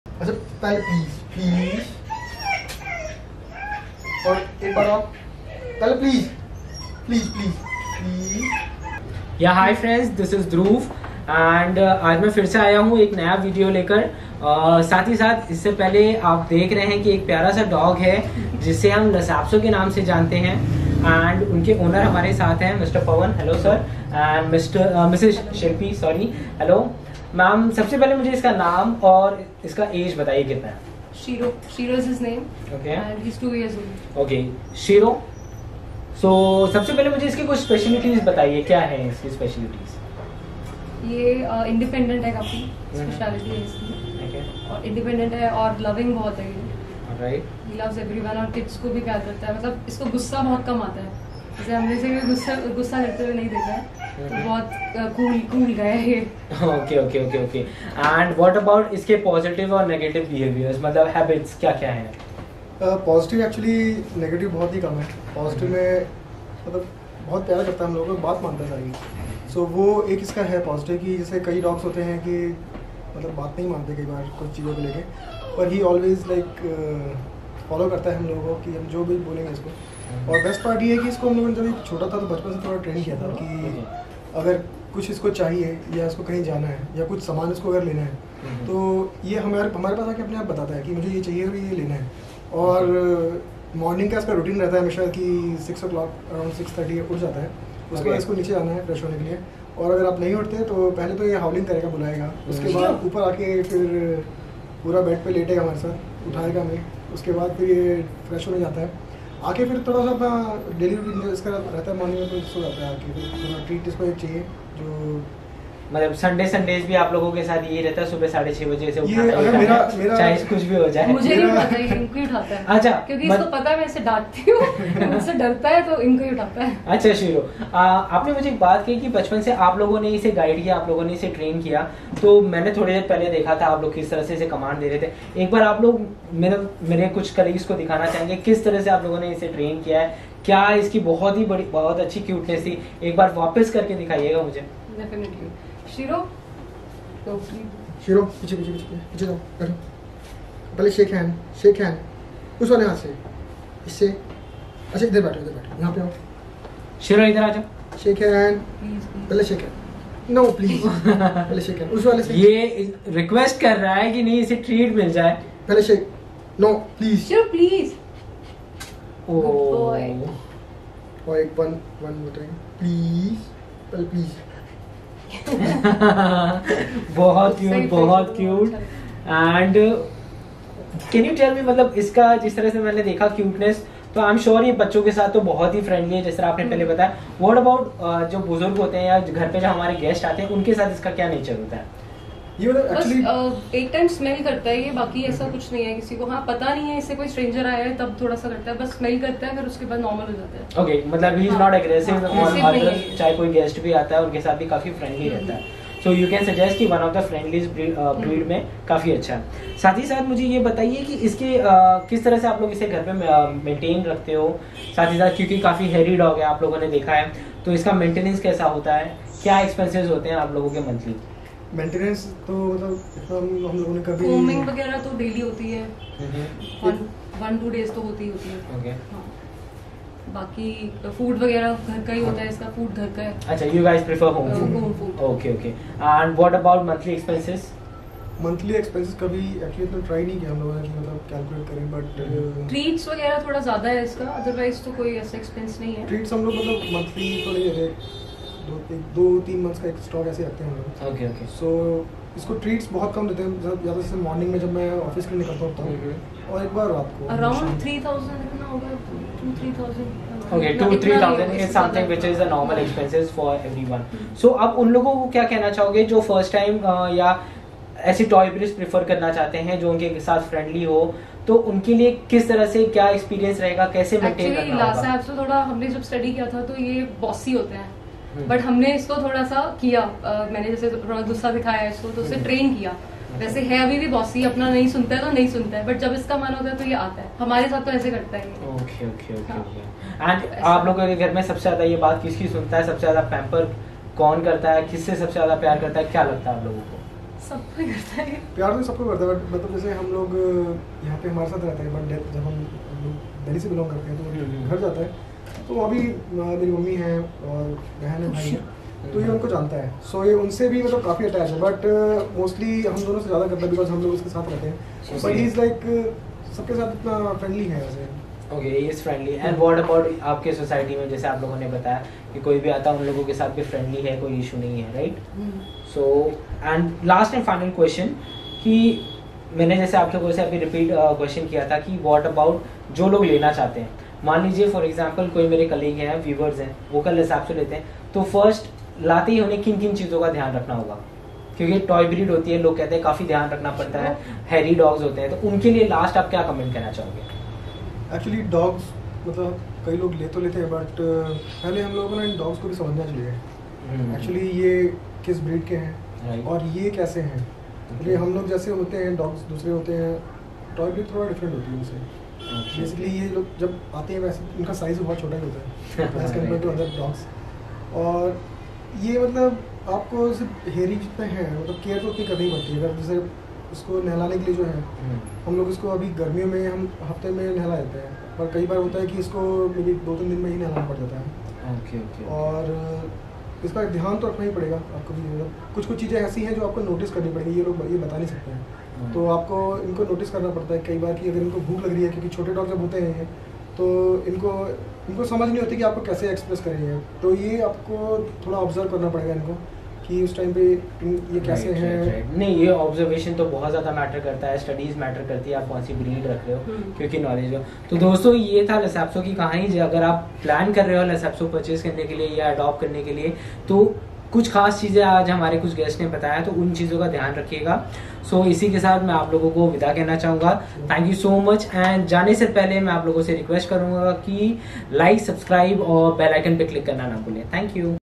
Lhasa Apso | मिलिए शिरो से (cutest lhasa apso ever)
in this video we talked about pros and cons of getting a Lhasa apso as a pet , we asked our guests following questions -
1- positive and negative traits of lhasa apso .
2- how good lhasa apso are with kids and elders ?
3-is lhasa apso bossy in nature ?
4-how much is the average monthly maintenance of lhasa apso ?

Lhasa apso dog breed
interview with dog parents